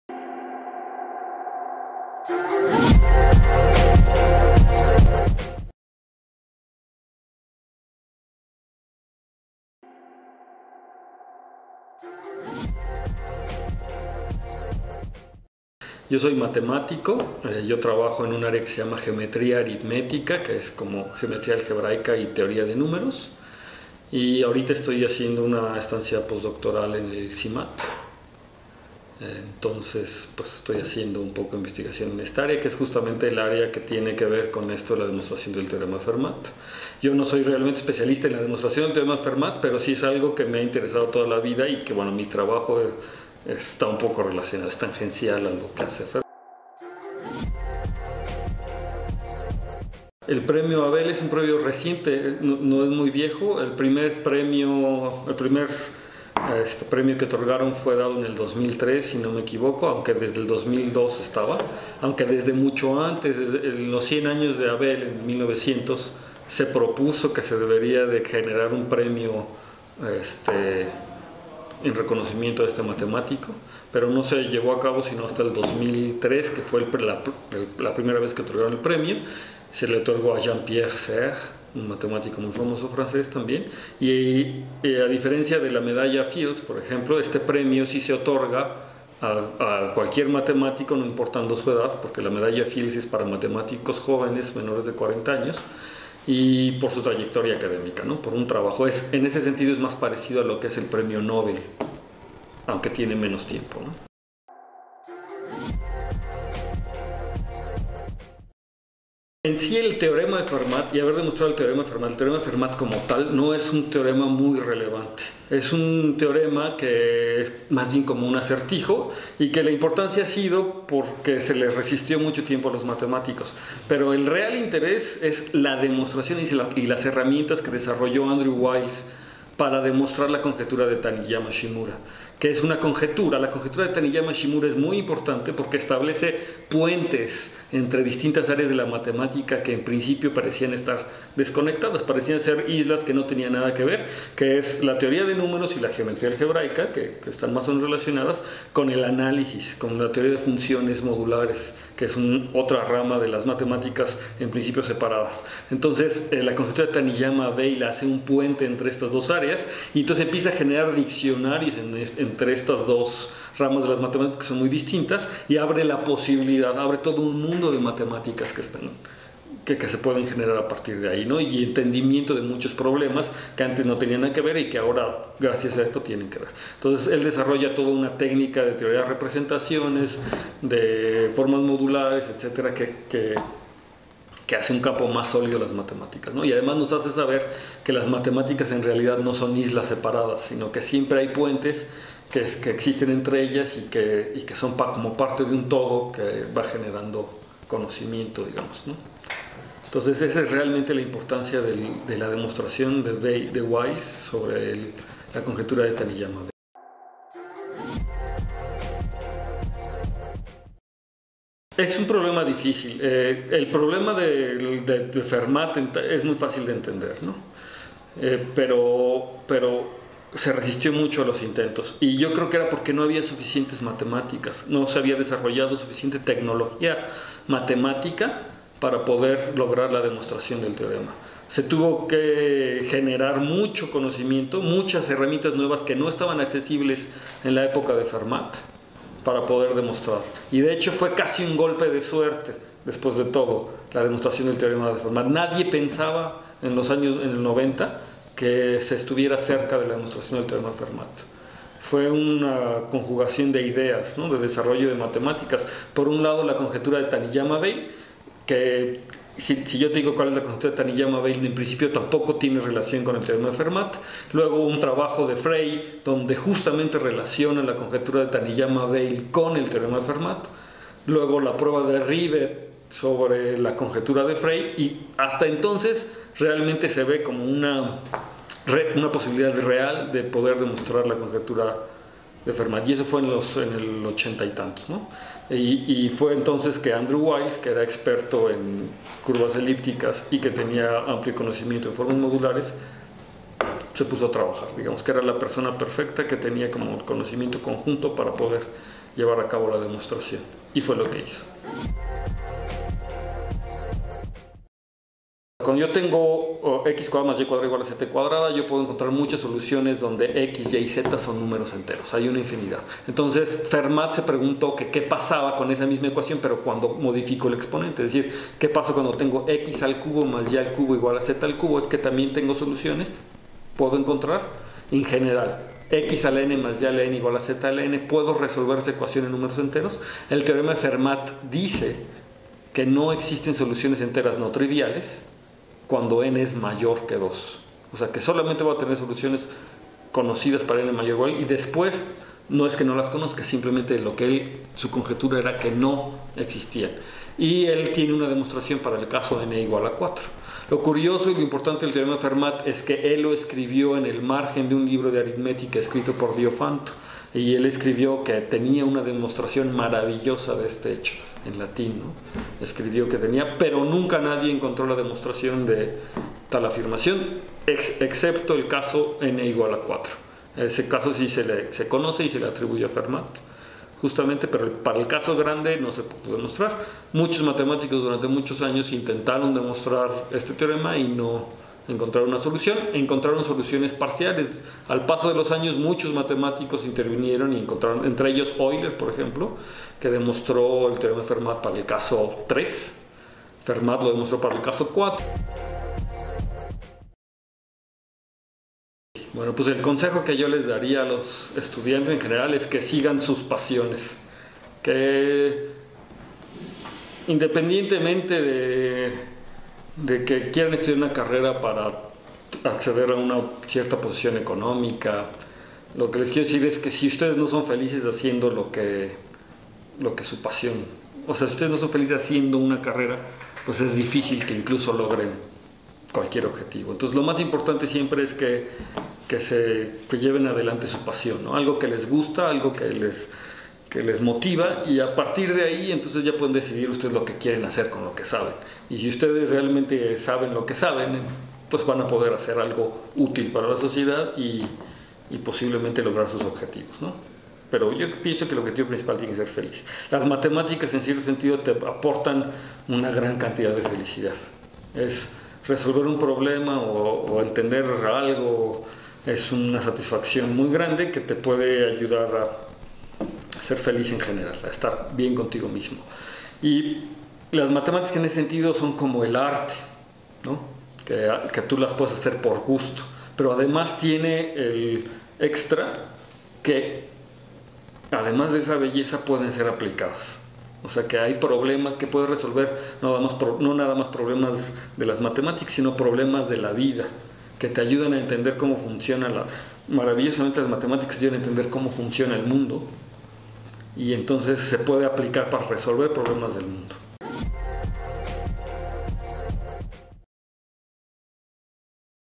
Premio Abel 2016 a Andrew Wiles. El Último Teorema de Fermat. Conversación con el Dr. Rogelio Pérez.
Premio Abel 2016: Andrew Wiles, por la demostración del último teorema de Fermat

Conversación con Rogelio Pérez Buendía

Video proporcionado por Ciencias TV, Serpente Films

Agradecemos el apoyo de